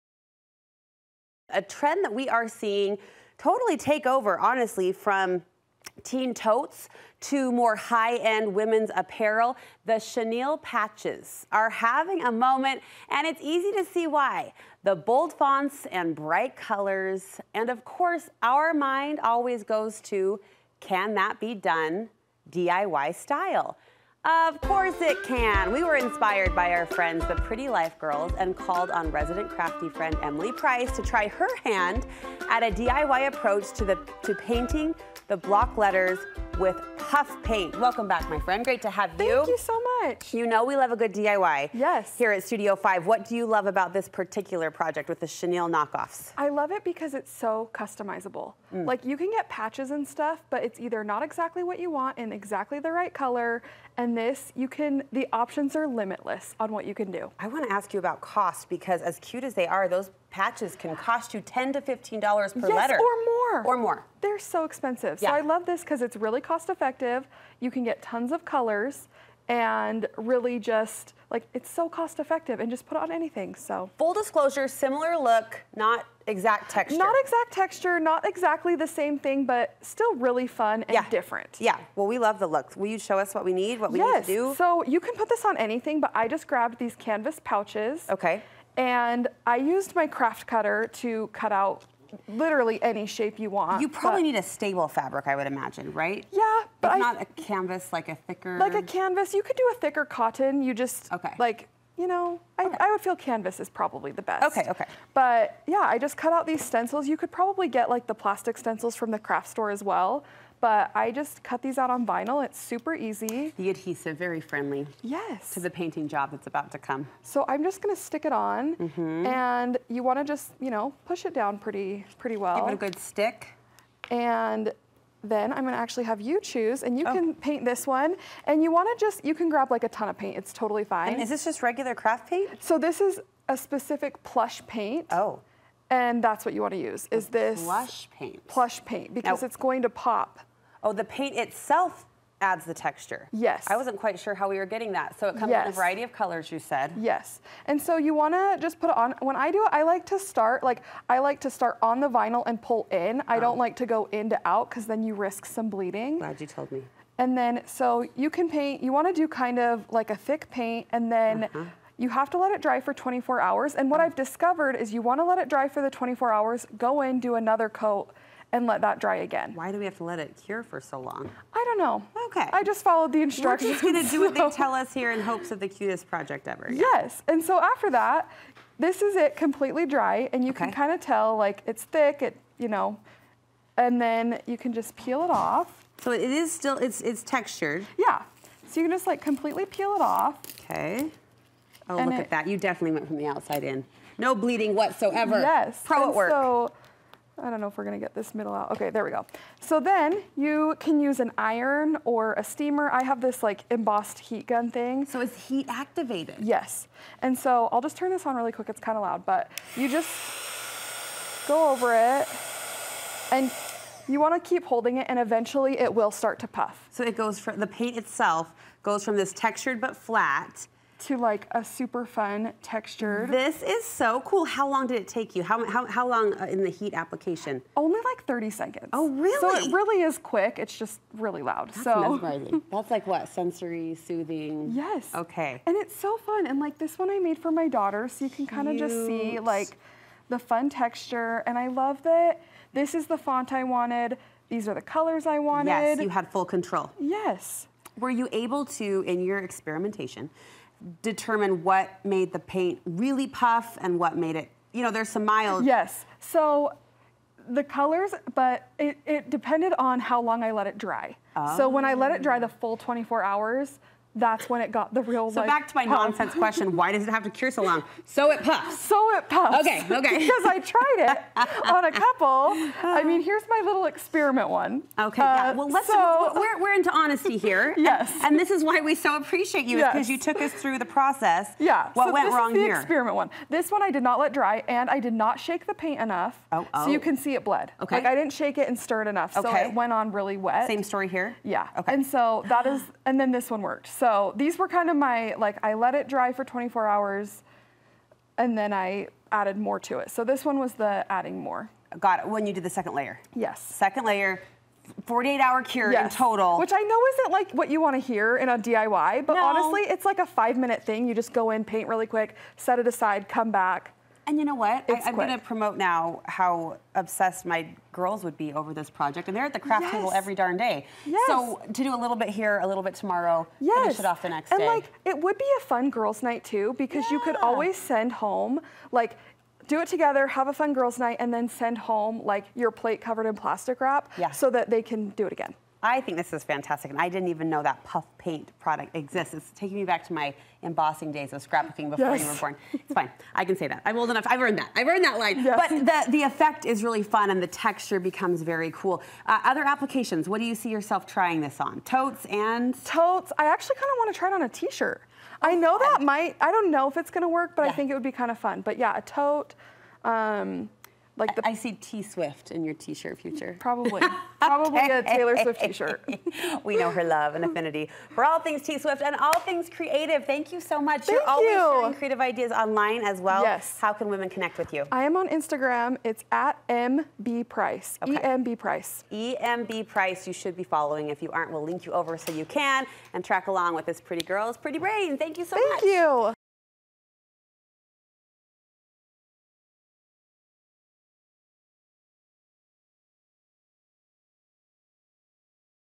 Make these trendy patches with puff paint! Here's the easy tutorial...
*You can use these trendy patches in all kinds of applications.*

It’s a trend we are seeing take over! From teen totes to high end women's apparel, chenille patches are having a moment, and we can see why! The bold fonts and bright colors  - and, of course, our mind always goes to... can that be done DIY?

Inspired by our friends, the Pretty Life Girls, Emily Price tried her hand at a DIY approach for these trendy patches by painting block letters with puff paint!

The DIY approach is an inexpensive and fun way to create the look for less. Get a group of teens (or moms!) together to create. Share the paints and a few bottles will go a long way. It’s also fully customizable as you choose the words and the colors.

You'll need a stencil, paint, brushes, and any blank material you want to personalize. Add the puff paint and heat to make the paint rise a bit and you have the look of a chenille patch you created yourself.

Follow along with Emily on Instagram, @embprice.

Thumbnail courtesy of: Palmy Pineapple